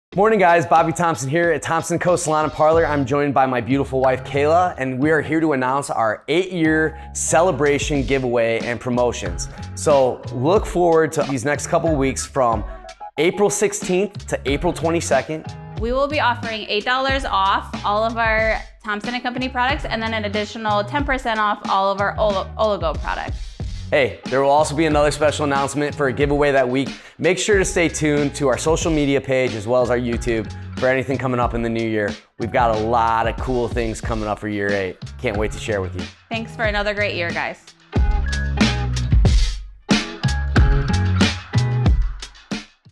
Thompson & Company Salon Parlor: 8th Year Anniversary!
And just like that...we turned 8! TIME TO CELEBRATE! Come join us as we celebrate our 8th year Anniversary with these amazing product promotions. Make sure to stay posted all week long as we celebrate 8 years of TCO. You never know what else we may have in store.